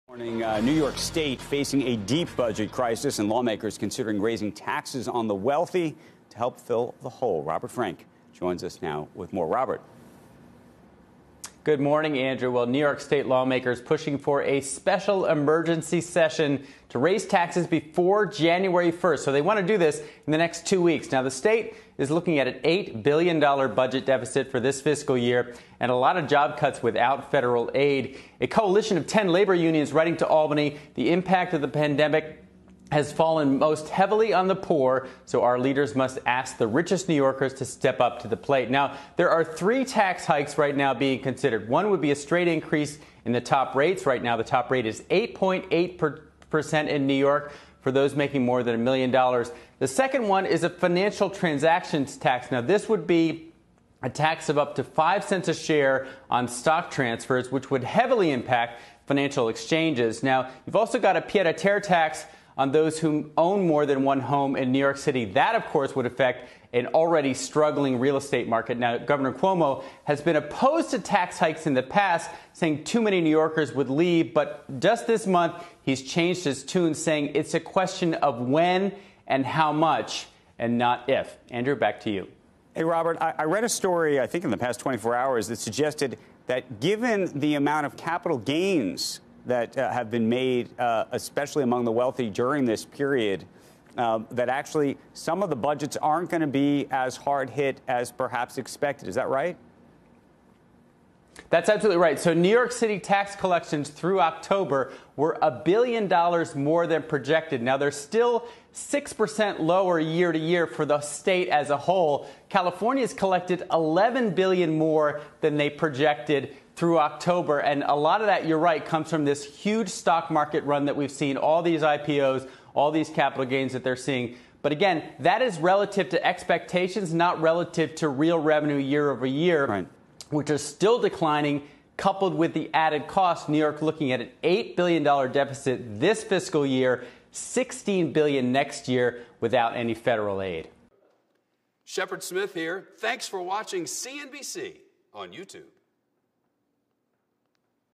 New York Governor Andrew Cuomo: Tax hike on the wealthy may be inevitable
New York state is facing a deep budget crisis and lawmakers are considering raising taxes on the wealthy to help fill the hole. CNBC's Robert Frank reports.